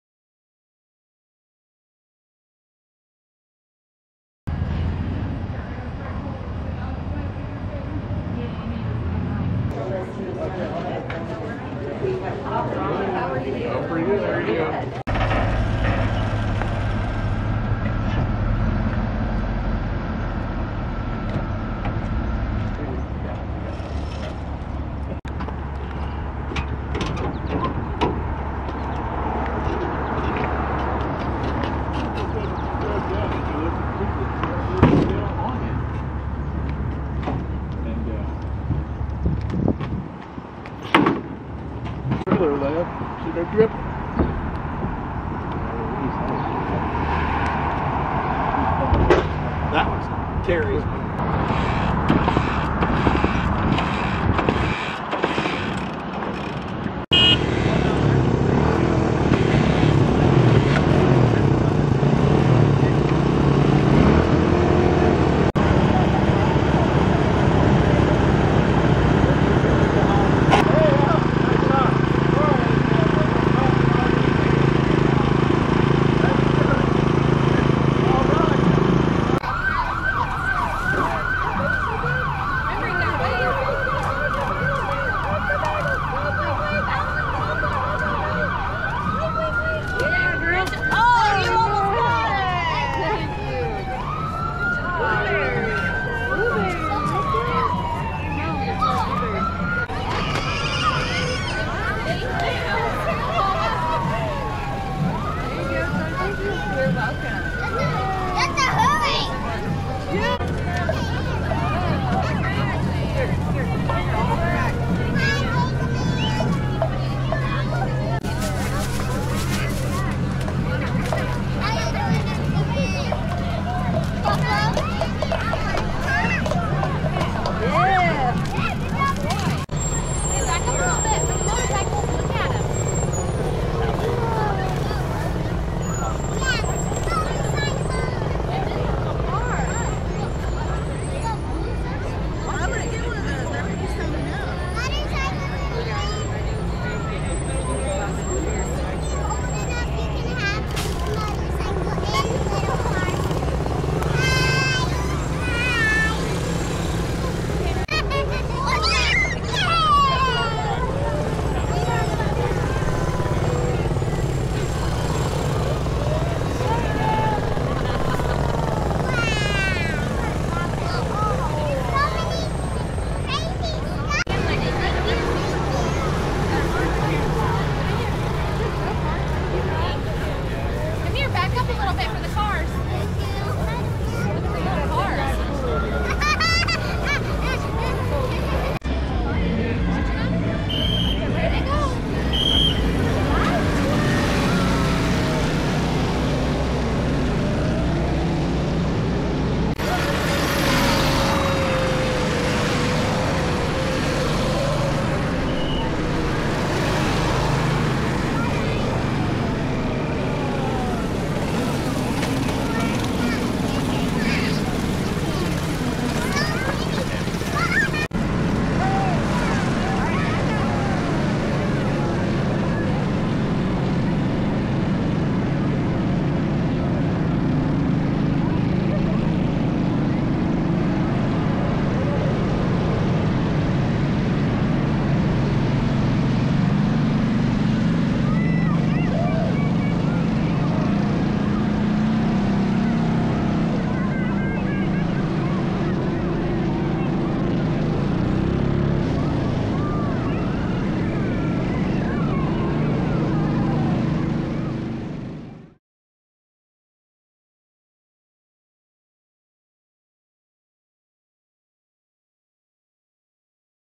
Bagelfest Parade
The Ansar Cycle Patrol and other Shriners units ride in the Bagelfest Parade in Mattoon, Illinois, on July 20, 2024.